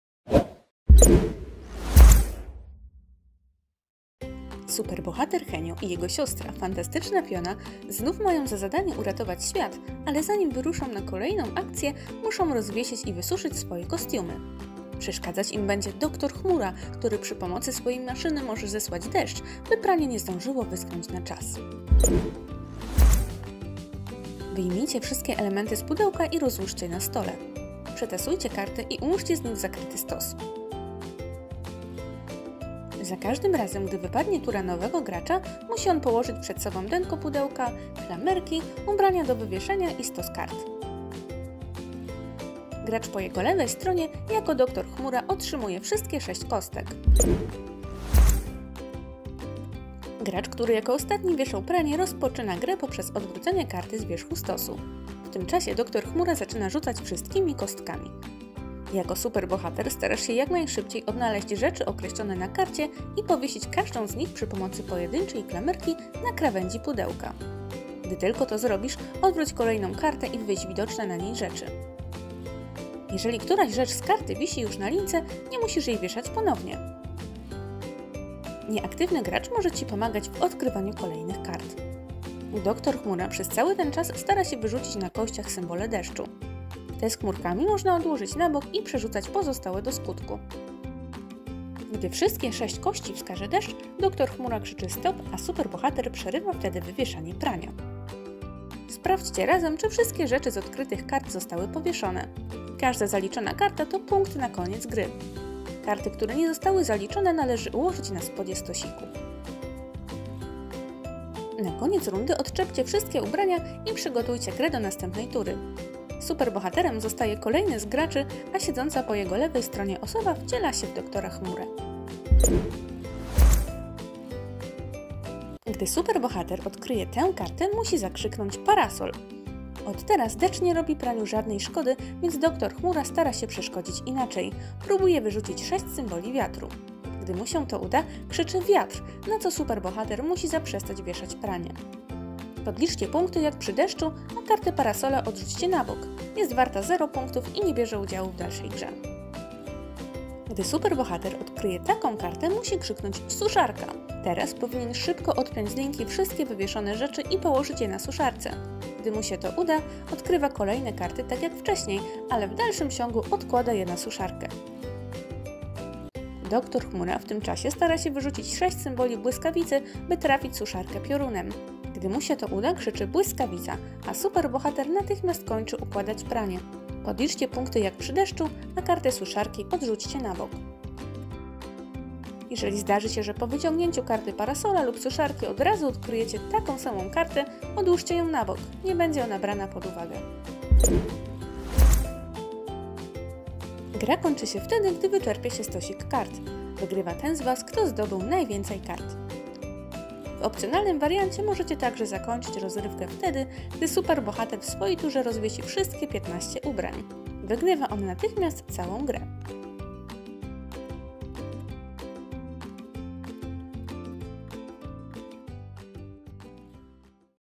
Granie w pranie - Zasady gry
Superbohater Henio i jego siostra Fantastyczna Fiona znów mają za zadanie uratować świat. Jednak zanim wyruszą na akcję, muszą wyprać i wysuszyć swoje kostiumy. Wydaje się to łatwe, ale trzeba się  spieszyć, gdyż Dr Chmura chce pokrzyżować im plany! Skonstruował maszynę, dzięki której manipuluje pogodą. Będzie wysyłał deszcz, wiatr i błyskawice, aby pranie nie zdążyło wyschnąć na czas! Bez swoich czystych kostiumów Superbohaterowie nie mają żadnych supermocy – a bez supermocy nie będą mogli uratować świata. Doktorowi Chmurze właśnie o to chodzi!

To dopiero jest zadanie: Trzeba w mig wysuszyć pranie!
Nie stój więc jak zmokła kura, Zbliża się deszczowa chmura!